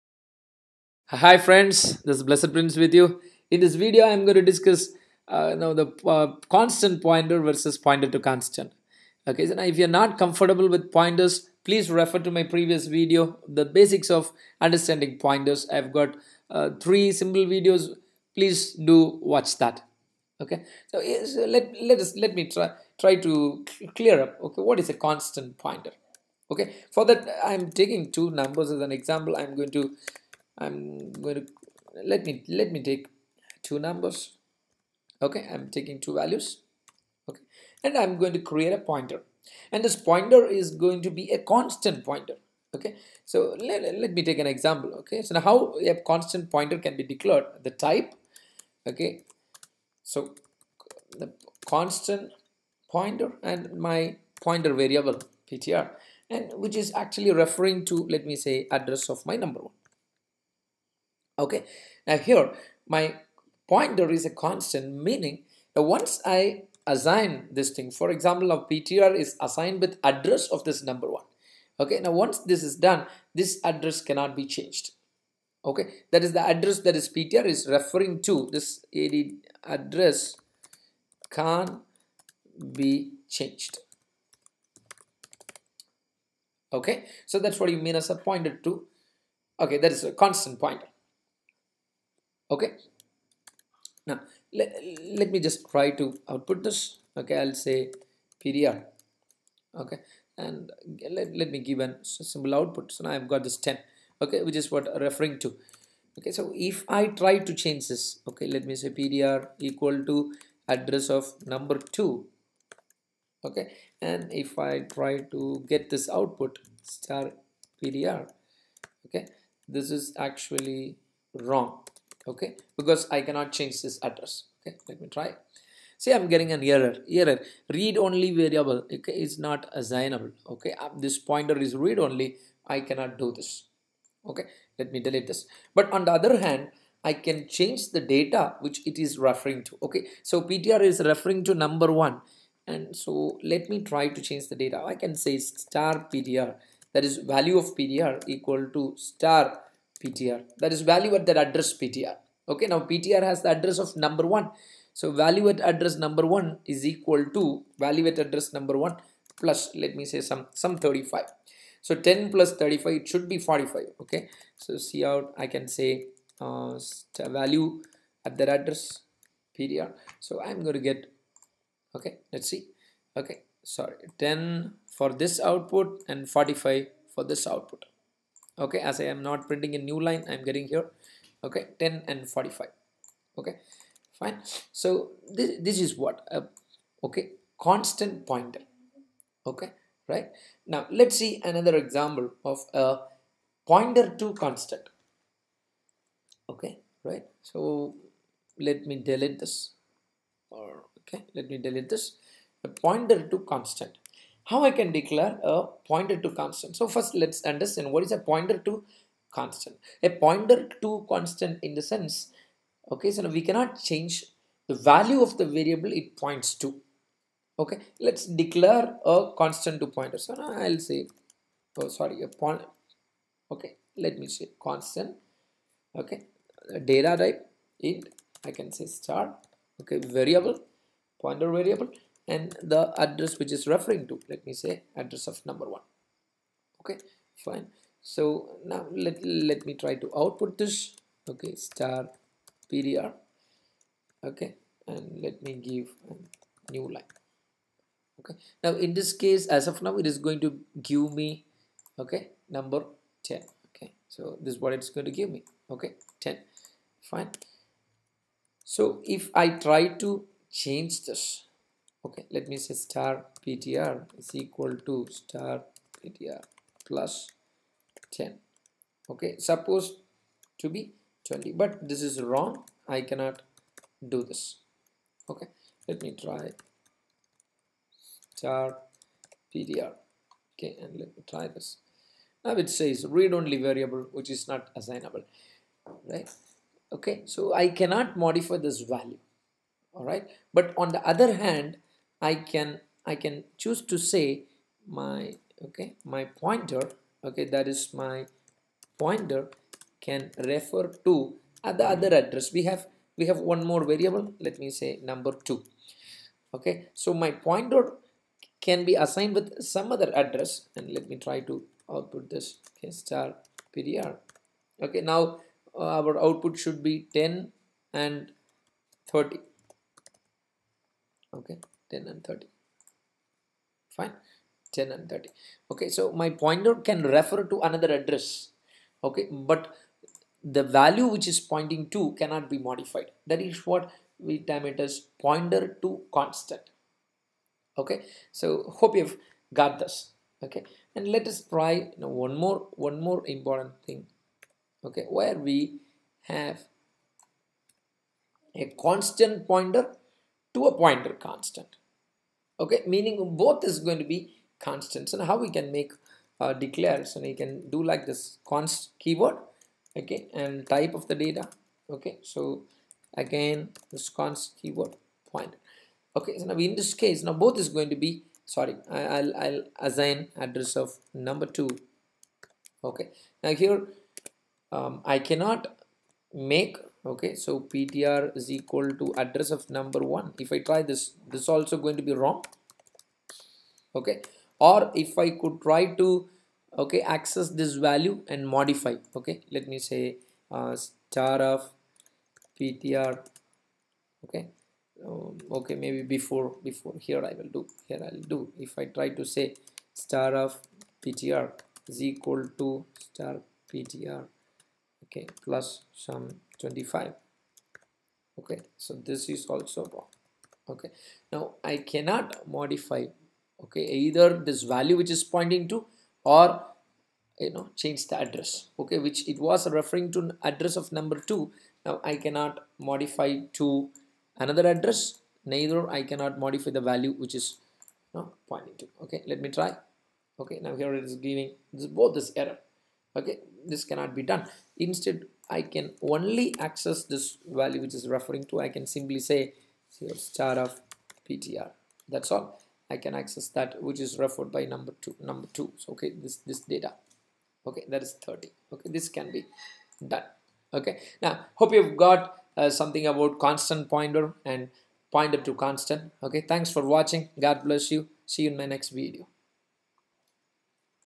constant pointer & pointer to constant with simple example
blessedprince This video helps you to understand constant pointer and pointer to constant